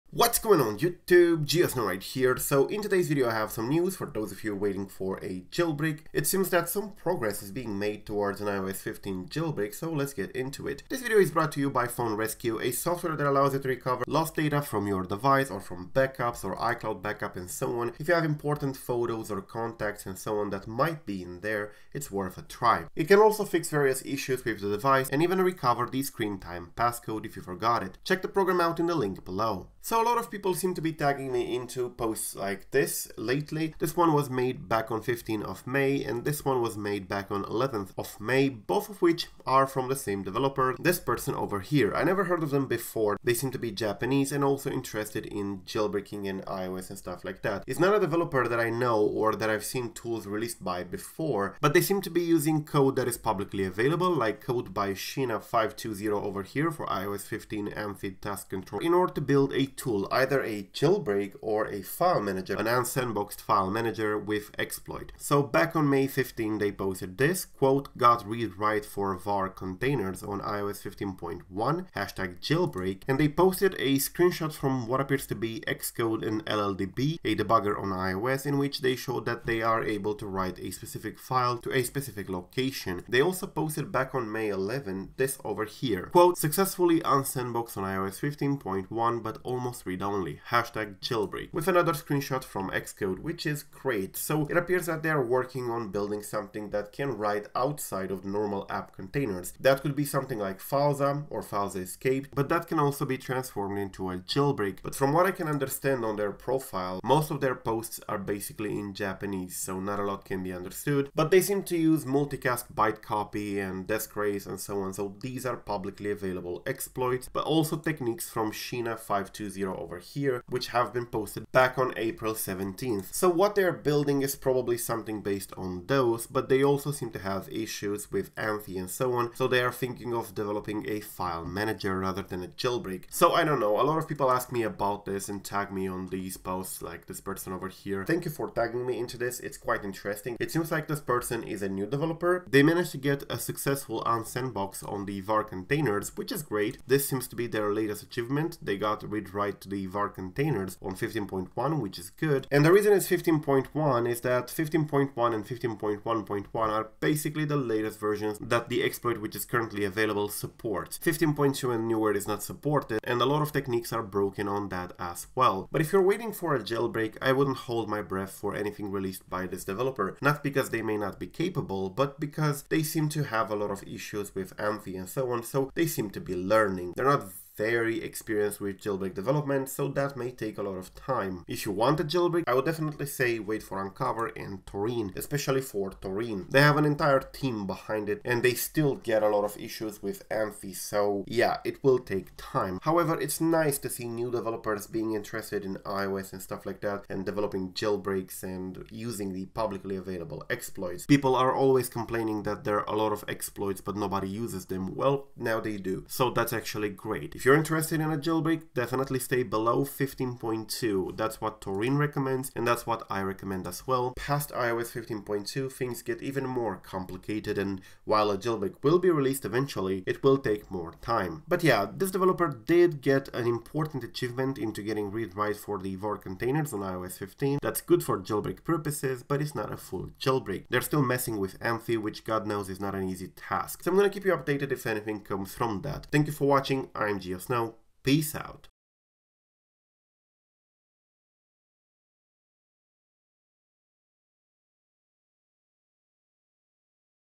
iOS 15.0 - 15.1.1 JAILBREAK News: New Developer Successfully Achieves R/W Unsandboxed Access
Today, we're discussing the achievement of a relatively new jailbreak developer in our community who has decided to tackle the iOS 15 jailbreak challenge and managed to obtain R/W access to /var/containers using publicly available exploit and code. This is a great start and according to the developer, they either plan to make an unsandboxed file manager or a fully working jailbreak.

It all depends on whether they manage to tackle AMFID which has been a great source of frustration and setbacks, not only for @akusio_RR, but also for established jailbreak teams like Taurine's Odyssey Team. If a jailbreak gets done, it would work on iOS 15.1.1 and lower for now.

~ GeoSn0w